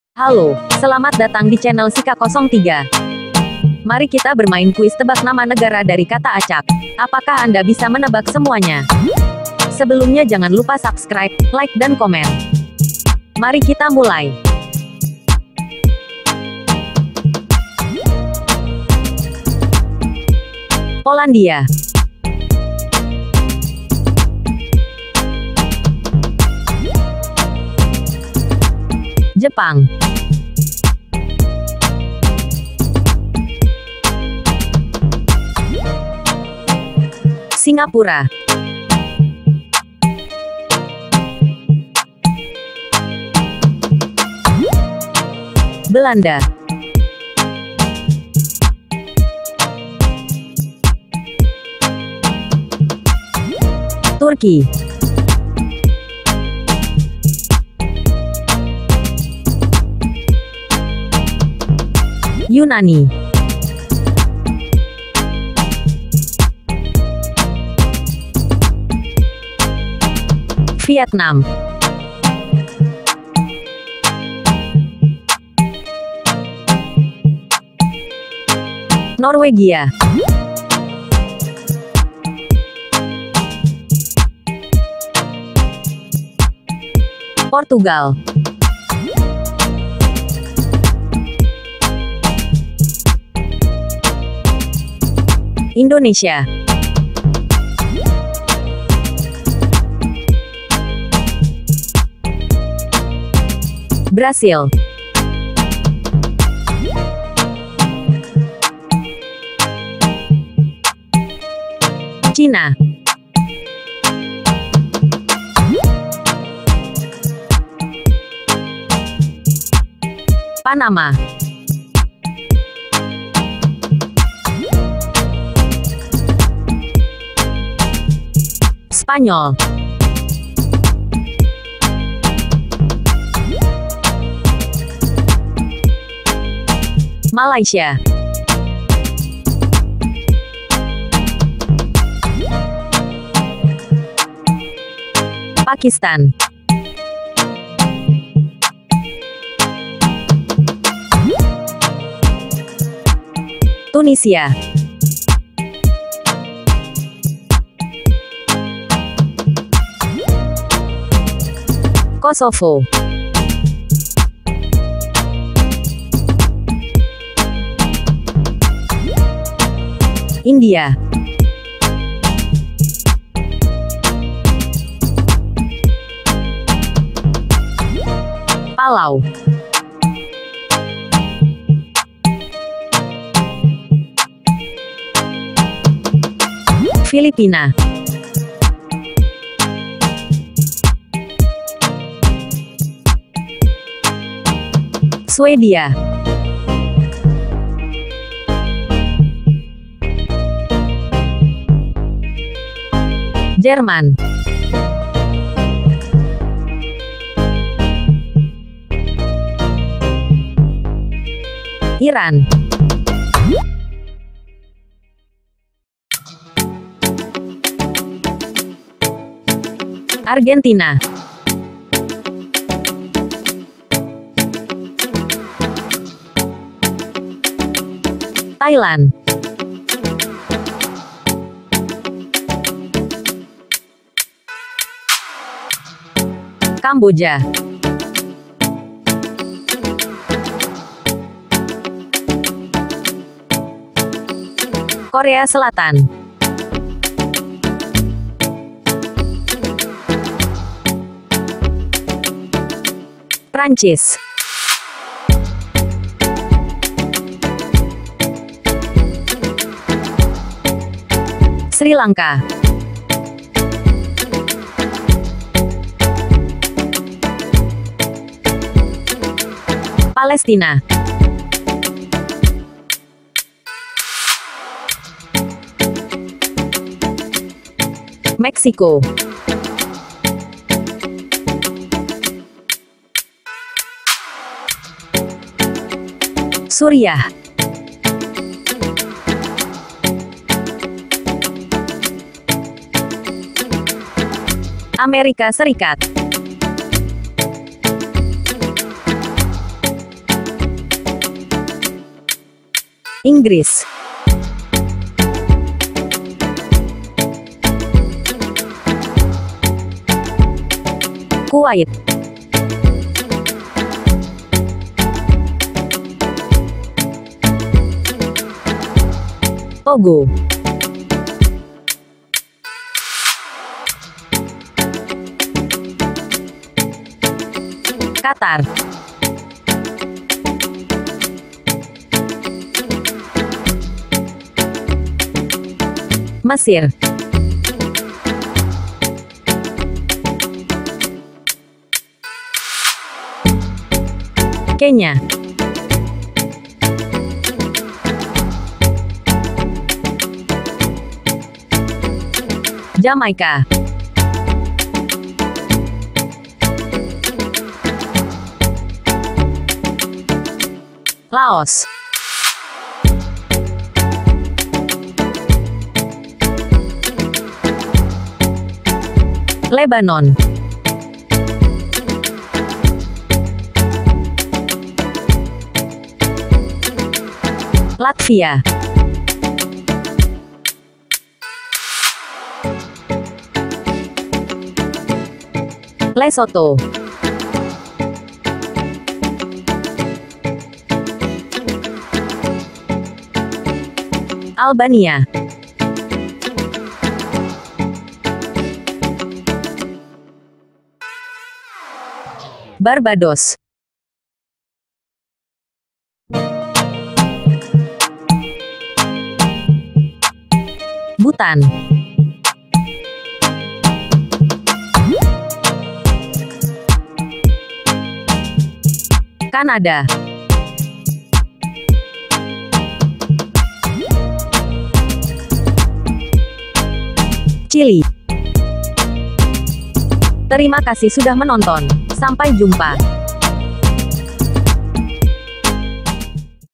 Tebak Negara Dengan Nama Acaknya | Kuis Negara
Tebak Negara Dengan Nama Acaknya | Kuis Negara @sikha03

Menebak negara dengan nama acaknya.
Apakah anda bisa menebaknya?
Ada 50 pertanyaan?
6 detik waktu menjawab

Semoga berhasil.